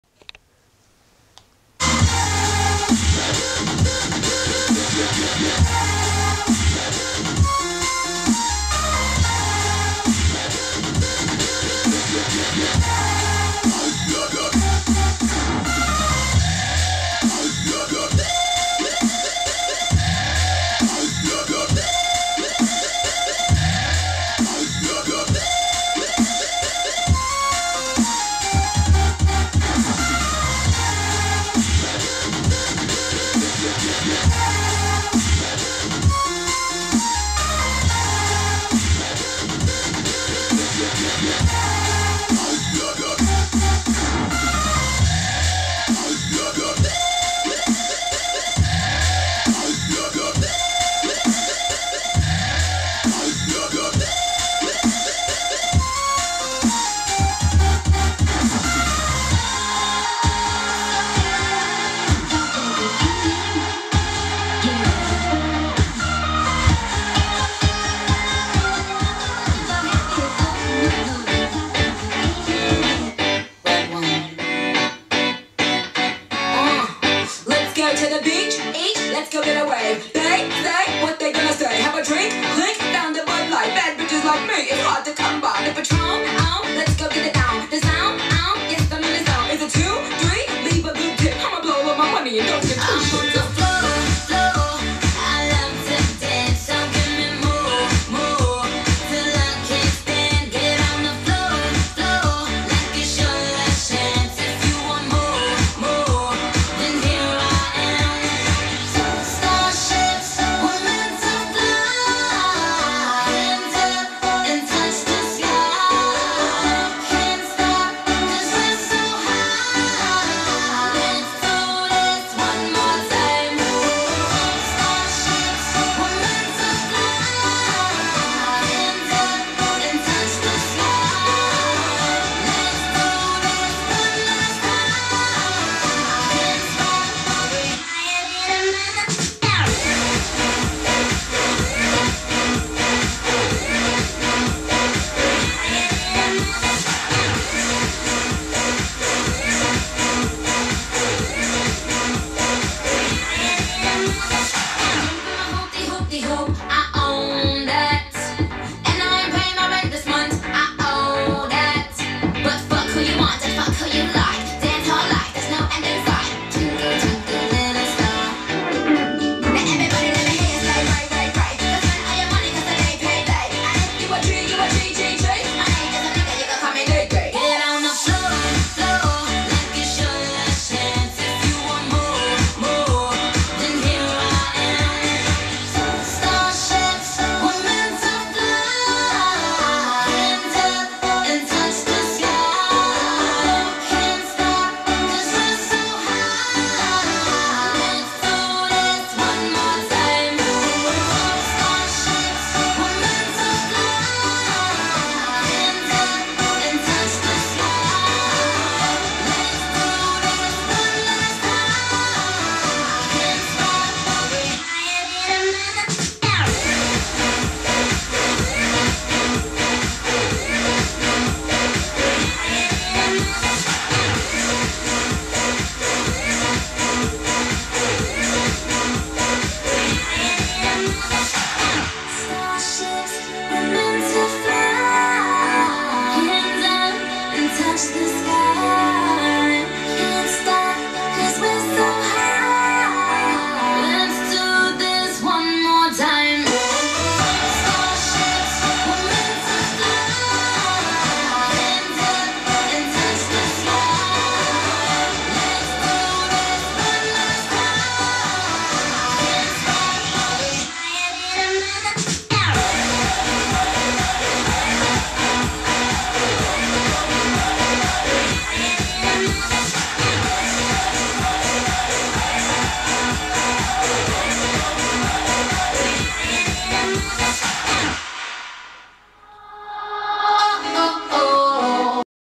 To Dom and Em
Home video with artworks and stained glass animations with the music of Aviichi and Nikki Minaj feat Em.